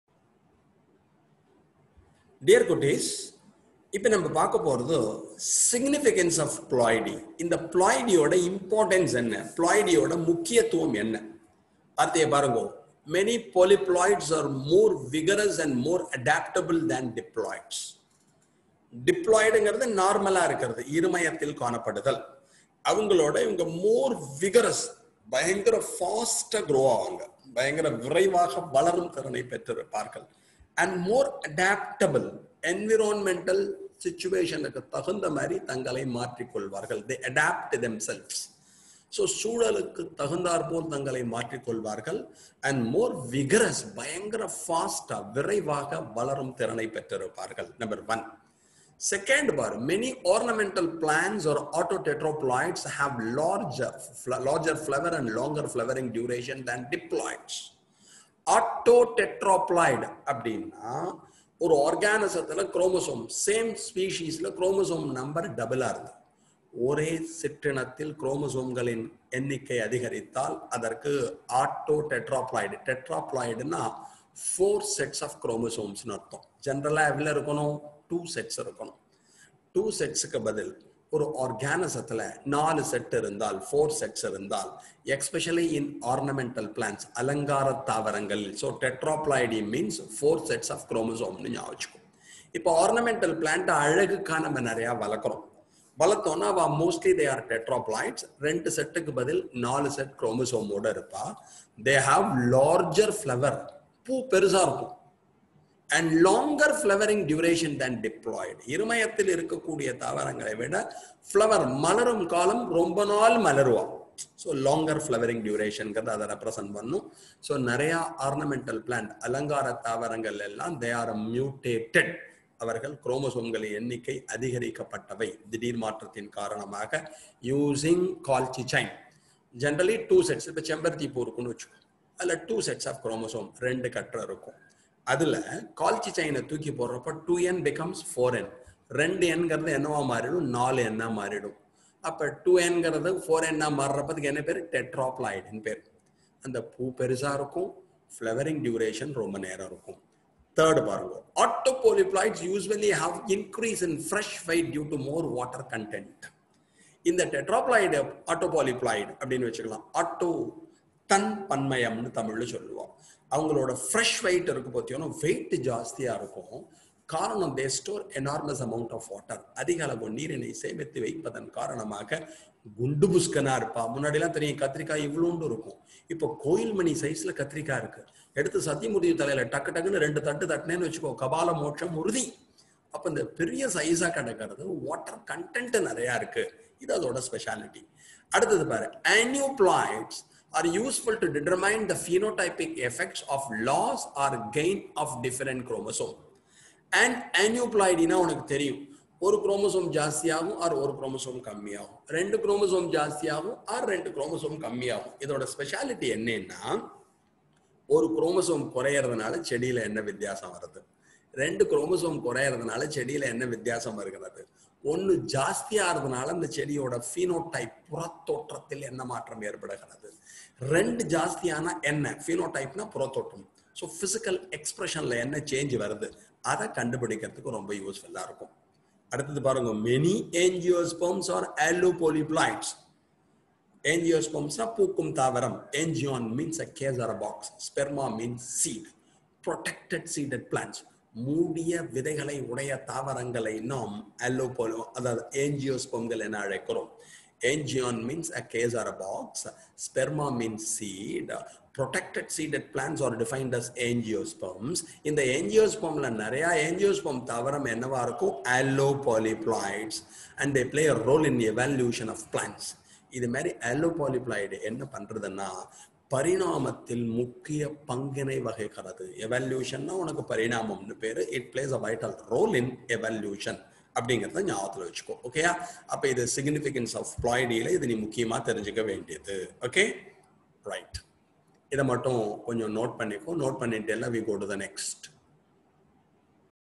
SIGNIFICANCE OF PLOIDY-பன்மயத்தின் முக்கியத்துவம்
Done by a well known biology teacher from the knowledge hub of Trichy, Tamil Nadu.
PLOIDY-SIGNIFICANCE